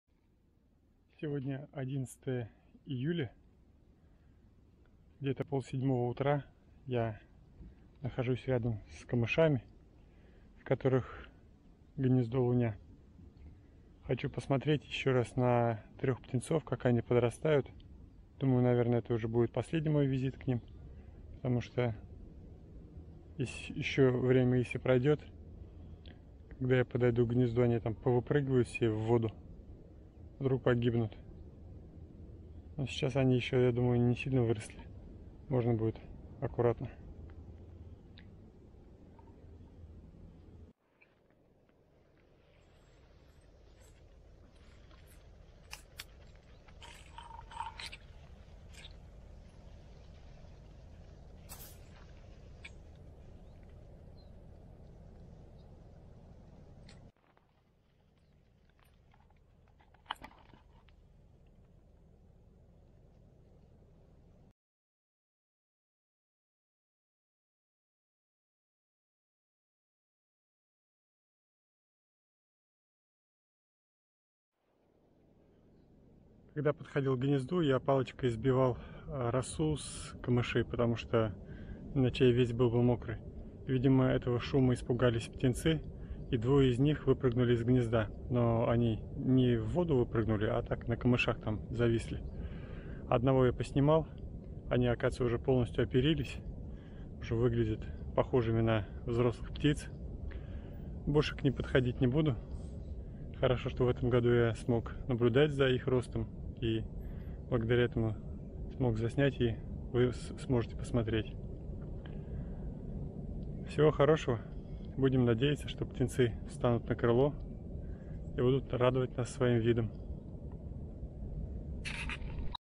Фотоохота с Альбертом.Птенцы болотного луня выросли.Эпизод пятый ,заключительный.
Съёмка 11 июля, в Чишминском районе.